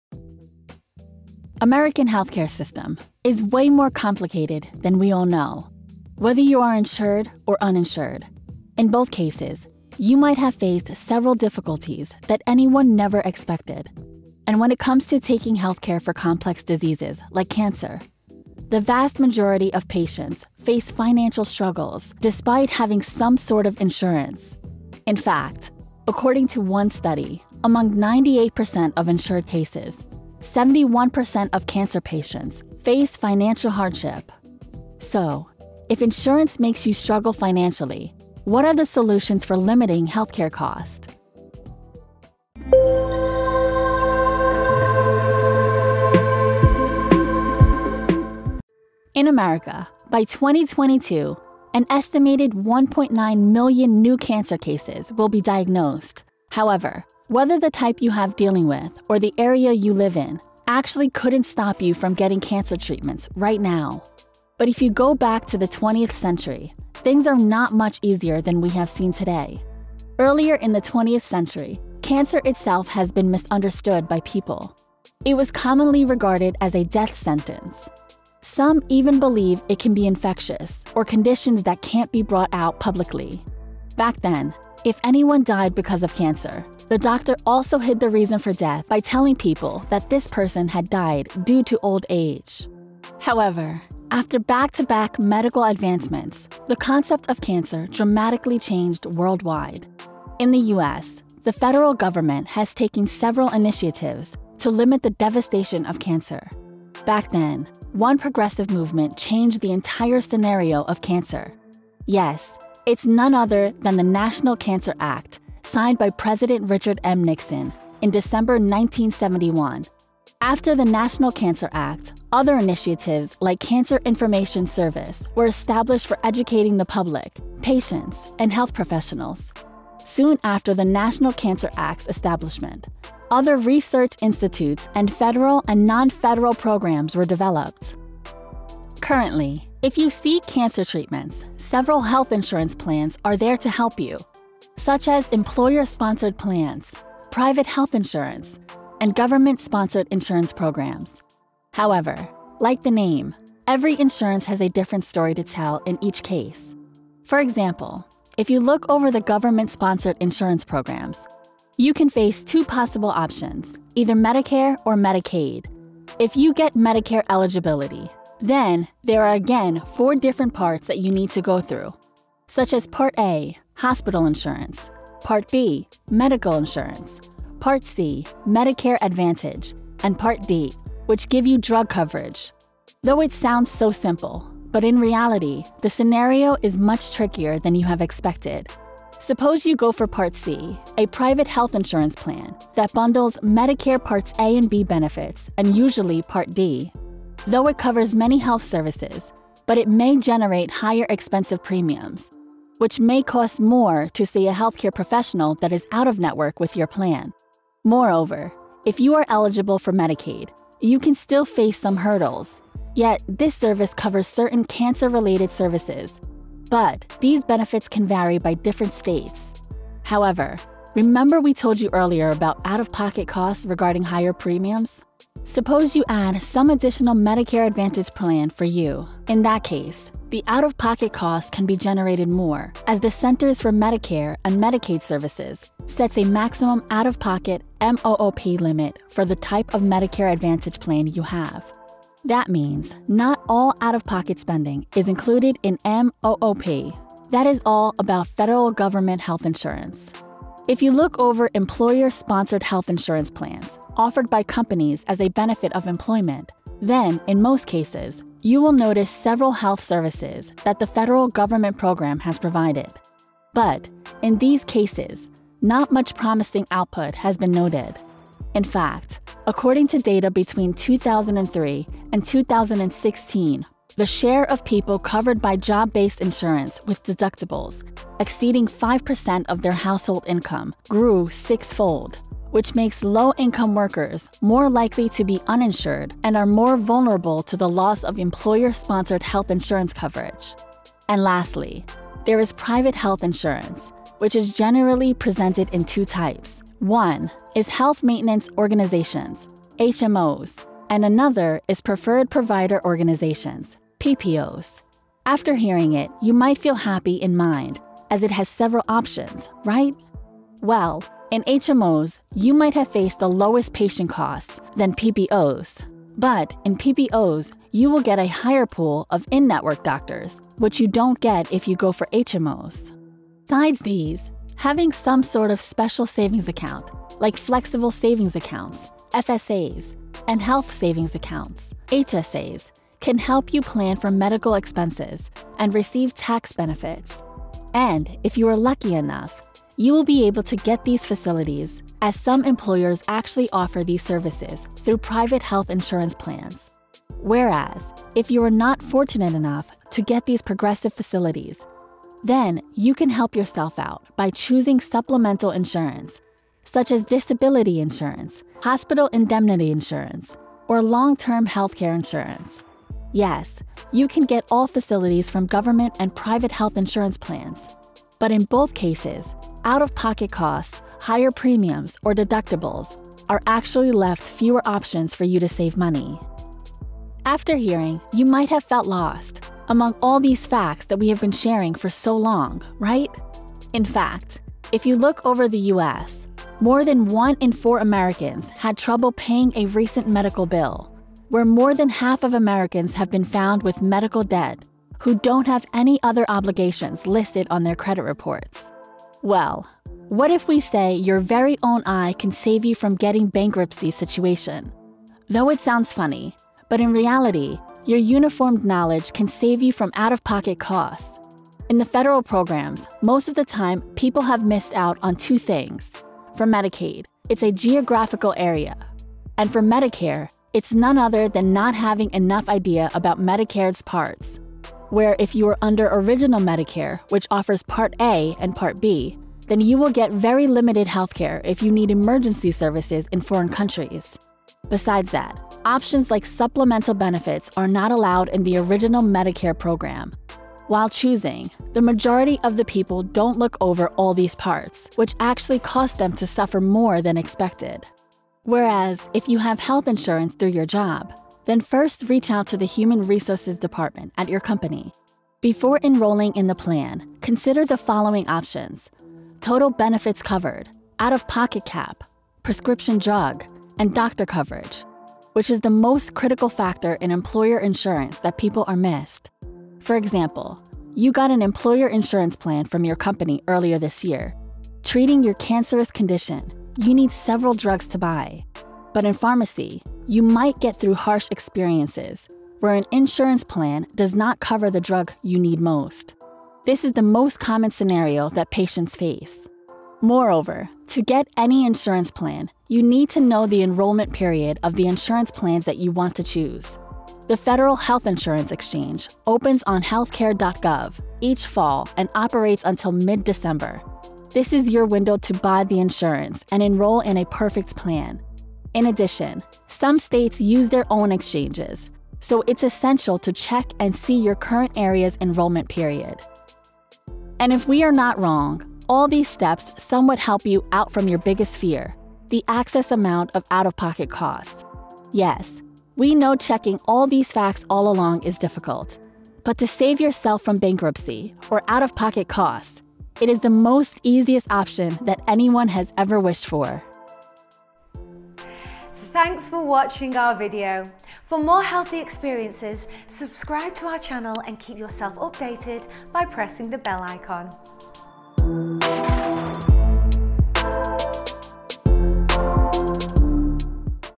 Best Health Insurance Plans for Cancer Treatment
When it comes to taking healthcare for complex diseases like cancer, most patients face financial struggles, despite having some sort of insurance! In fact, according to one study, among 98% of insured cases, 71% of cancer patients face financial hardship! So, if insurance makes you struggle financially, what are the solutions for limiting healthcare costs.
00:00 - Intro
00:48 - Cancer in US
02:18 - Types of Insurance
06:16 - Solution For Out Of Pocket